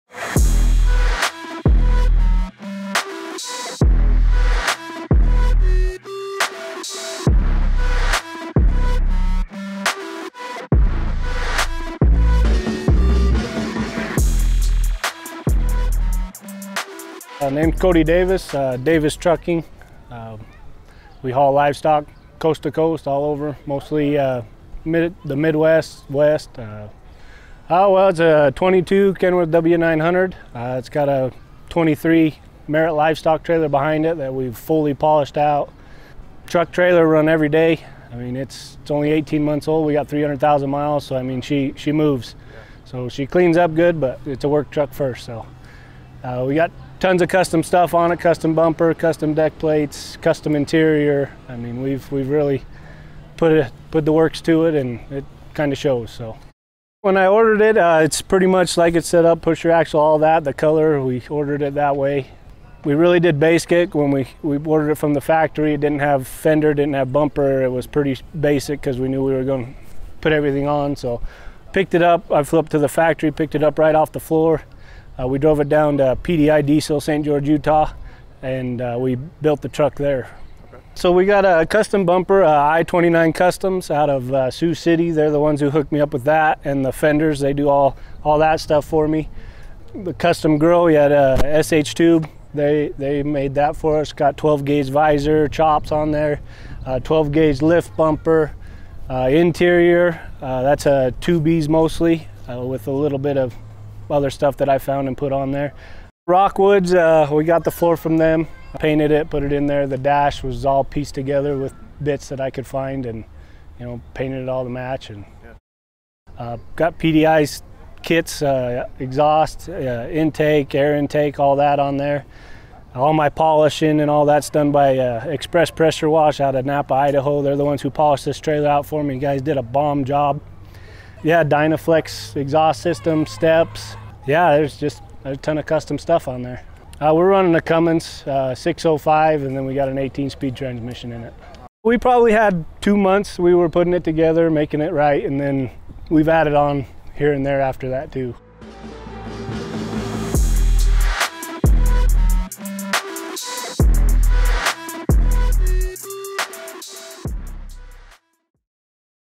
Livestock-hauling owner-op’s 2022 Kenworth W900L
Cokeville, Wyoming-based Cody Davis, owner of three-truck Davis Trucking LLC, hauls livestock across the U.S. with this 2022 Kenworth W900L and accompanying 2023 Merritt livestock trailer. Davis showed the combo at the 2023 Shell Rotella SuperRigs working truck show in Gillette, Wyoming, where it won in three categories — the Tractor-Trailer Division, Best Chrome, and Best Lights. Davis’ rig also won the People’s Choice Award after a public voting period.

Davis later in the year would go on to win in two categories of Overdrive's 2023 Pride & Polish competition

Catch plenty more in the way of custom and restored rigs in Overdrive’s Custom Rigs section of the